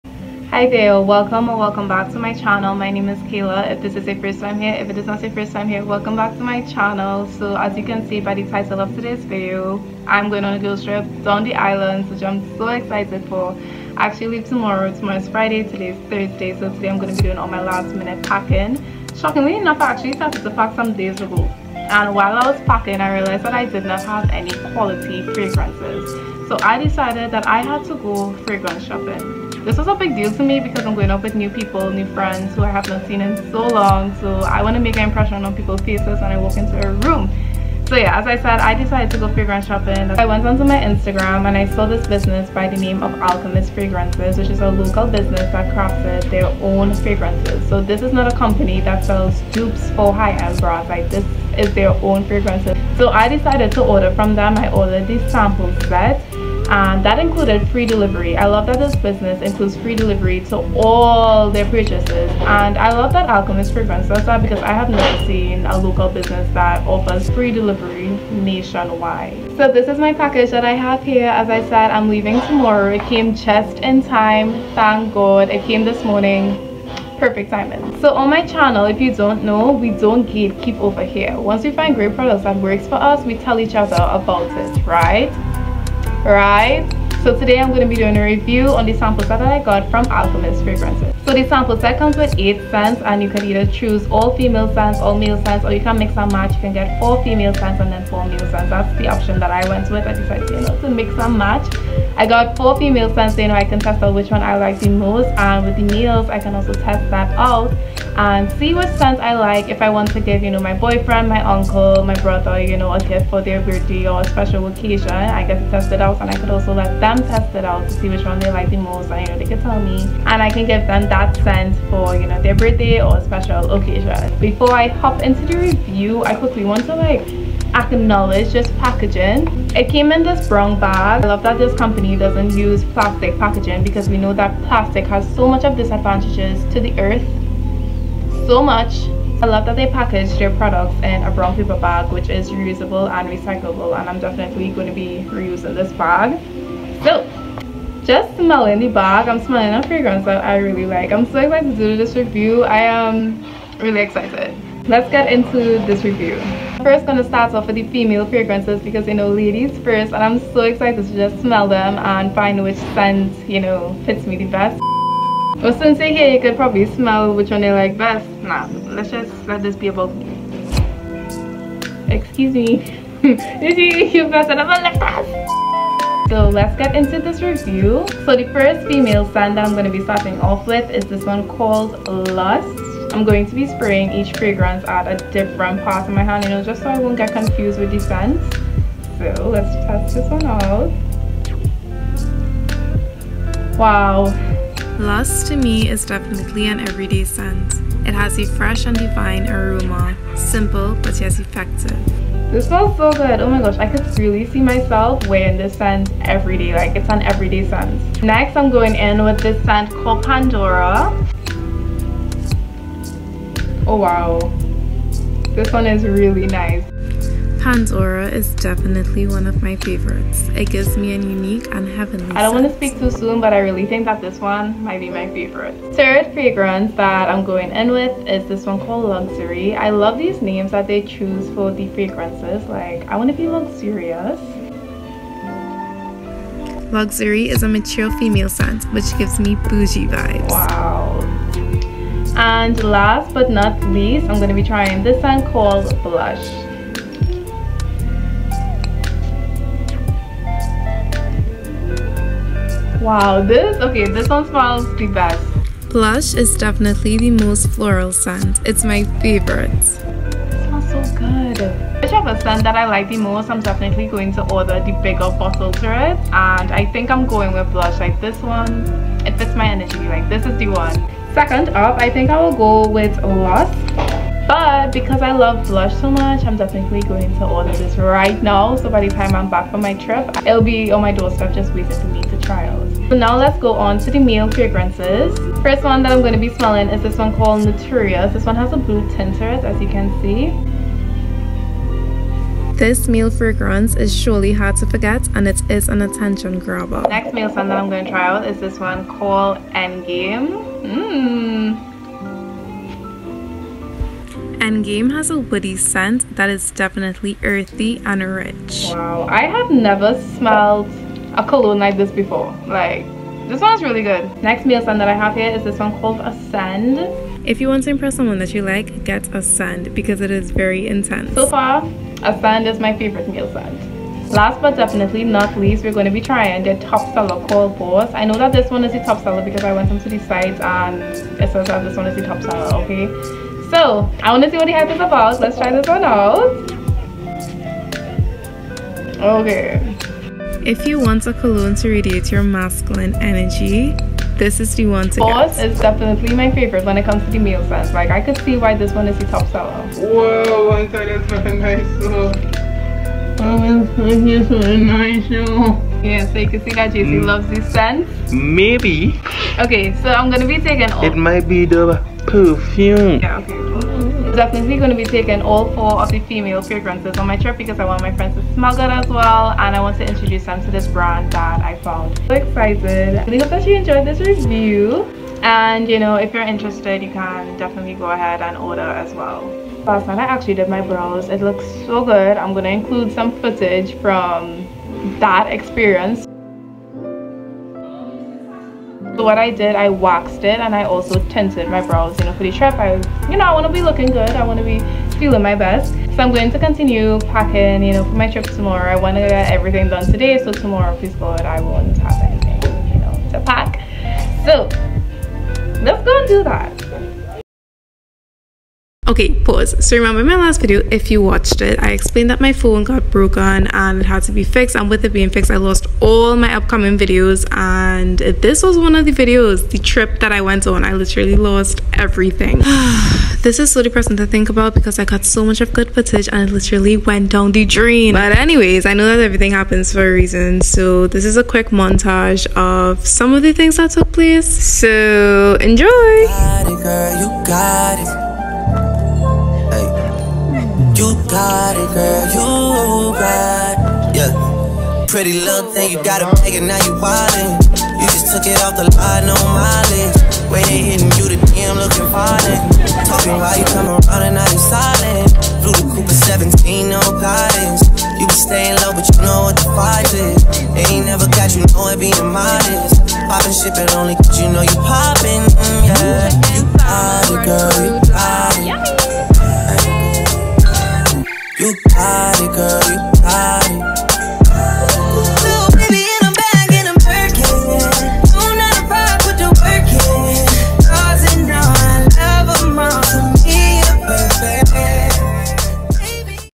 FRAGRNACE REVIEW AND DDI GIRLS TRIP | TRINIDADIAN YOUTUBER
How are you?!! Let me know if you're interested in trying Alchemist Fragrances and if you enjoyed the video.